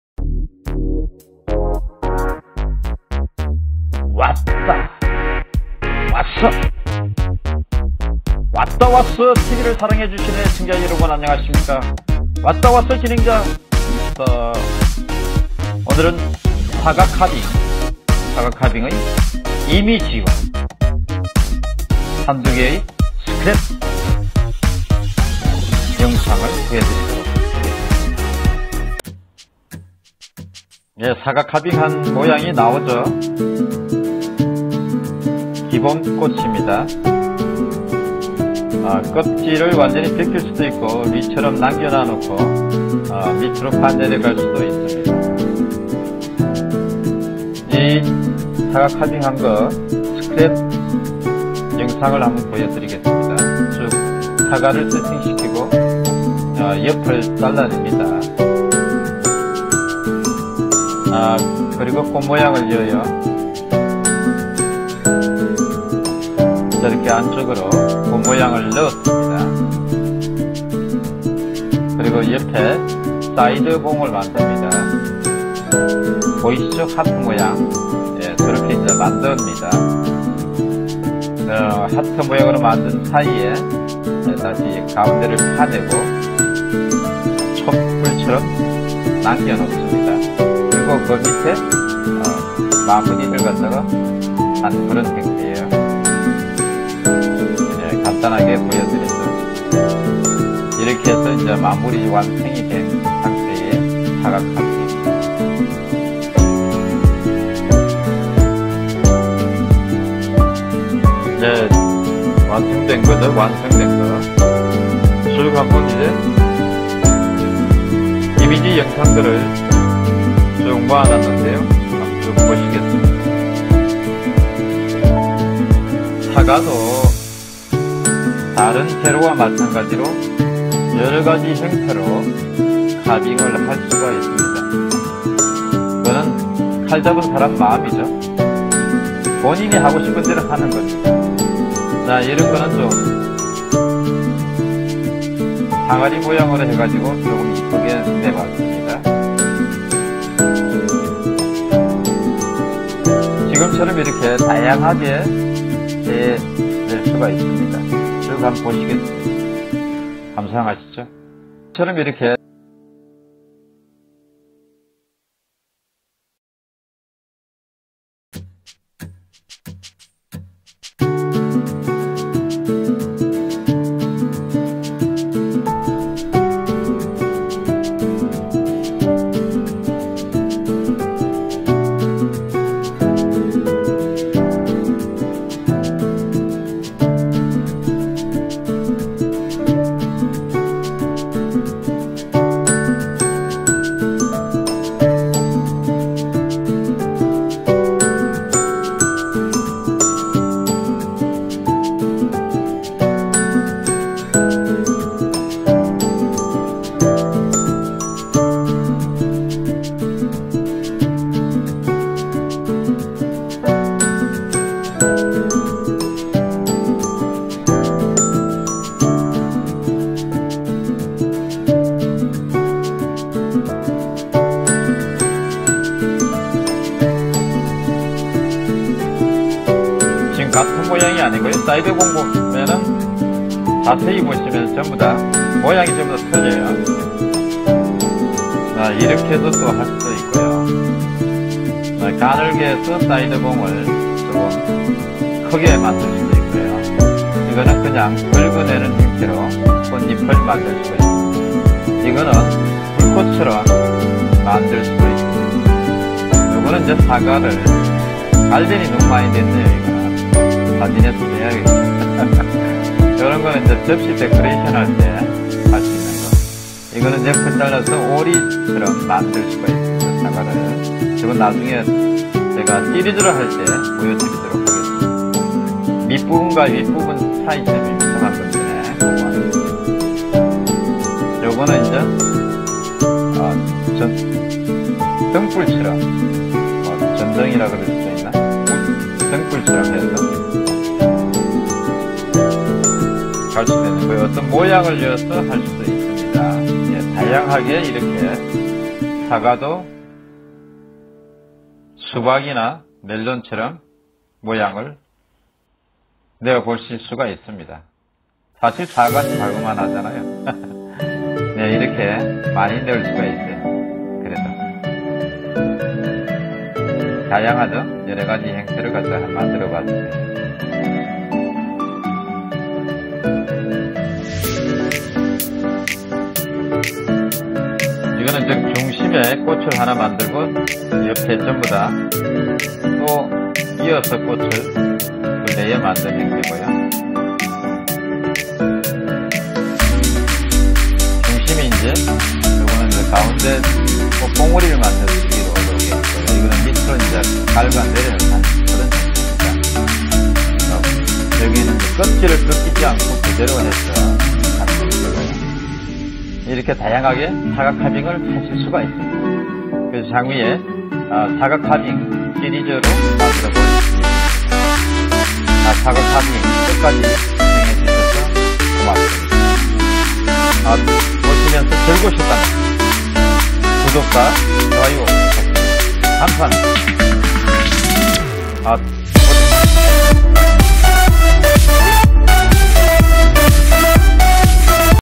carving.apple.apple carving.사과카빙.사과로 꽃 모양만들기 왔다왔어
사과를 재료로 다양한 모양을 만들수 있고그중에 몇가지만 만들어 보았습니다.
늘 재미있게 봐 주시길 바랍니다